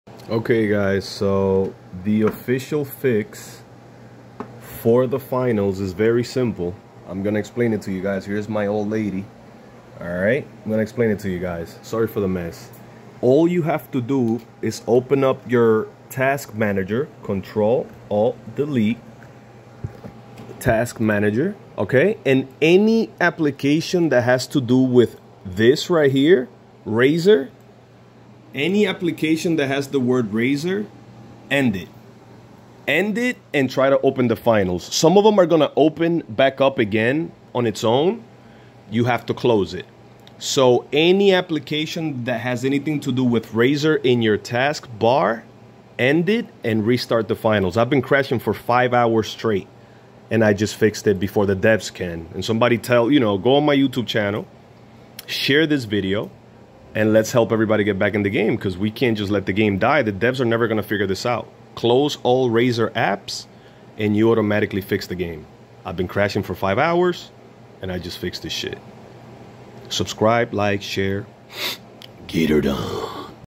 OFFICIAL FIX FOR THE FINALS UNREAL ENGINE 5 CRASH AFTER UPDATE 2.8 (SEE PINNED COMMENT!)
All you have to do is end all processes related to "RAZER" for example RAZER SYNAPSE OR GAMING RAZER SERVICES via task manager. Then reopen up the game, consider rechecking the processes because some open as soon as you close them, it takes a few tries, after all razer processes have been ended, try to run the finals and it will work.

Also consider changing your power mode to high performance not balanced or power saver.

OFFICIAL FIX FOR THE FINALS UNREAL ENGINE 5 CRASH AFTER UPDATE 2.8